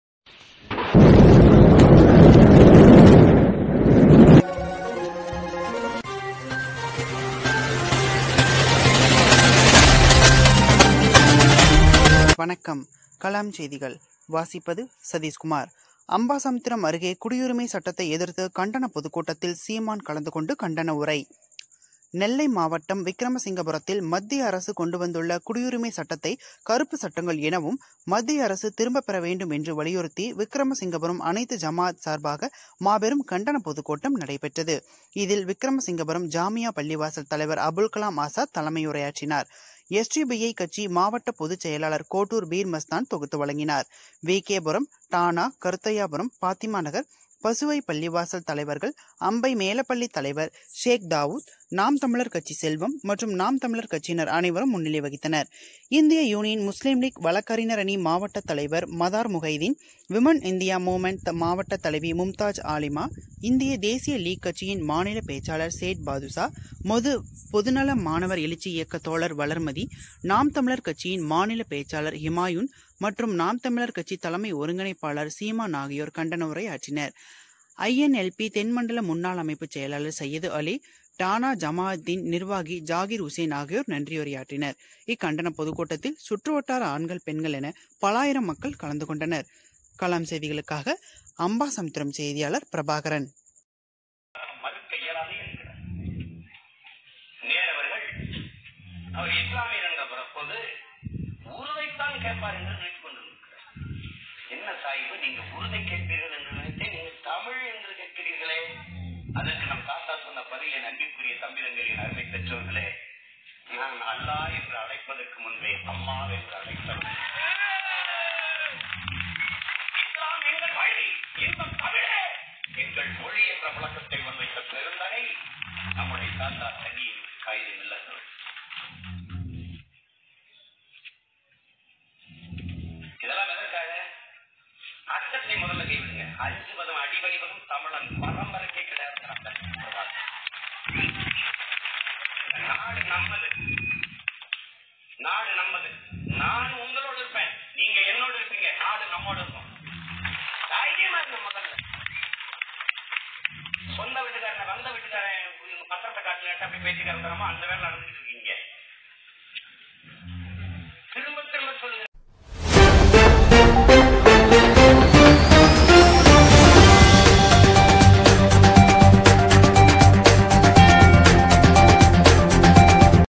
KALAM NEWS அம்பாசமுத்திரம் அருகே குடியுரிமை சட்டத்தை எதிர்த்து கண்டன பொதுக்கூட்டத்தில் சீமான் கலந்து
KALAM NEWS
அம்பாசமுத்திரம் அருகே குடியுரிமை சட்டத்தை எதிர்த்து கண்டன பொதுக்கூட்டத்தில் சீமான் கலந்து கொண்டு  கண்டன உரை.